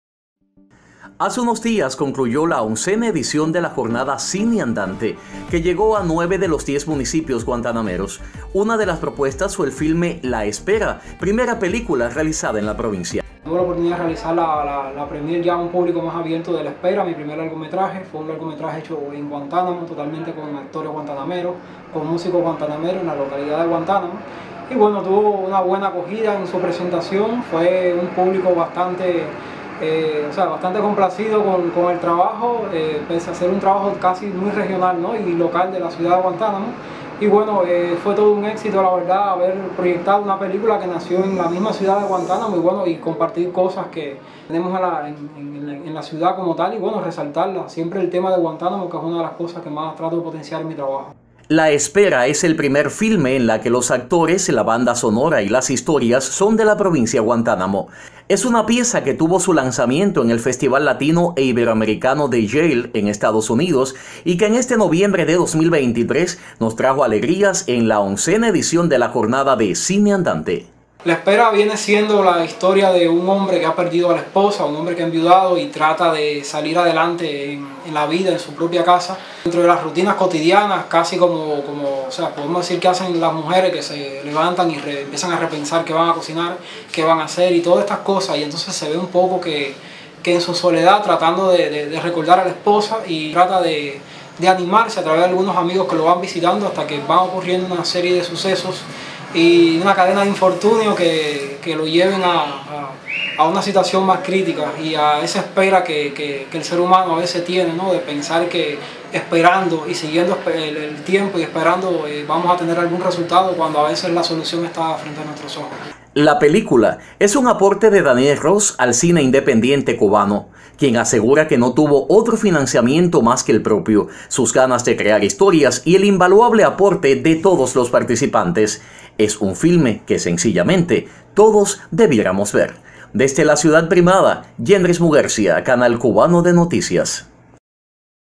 Presentan el largometraje "La Espera" en Baracoa
En el alto oriente cubano la realización audiovisual cada año es defendida desde una propuesta de la Asociación Hermanos Saíz, a través de la Jornada Cine Andante. Recién concluida la oncena edición, la presentación de la película "La Espera" fue uno de sus atractivos. Yendrys Muguercia nos informa.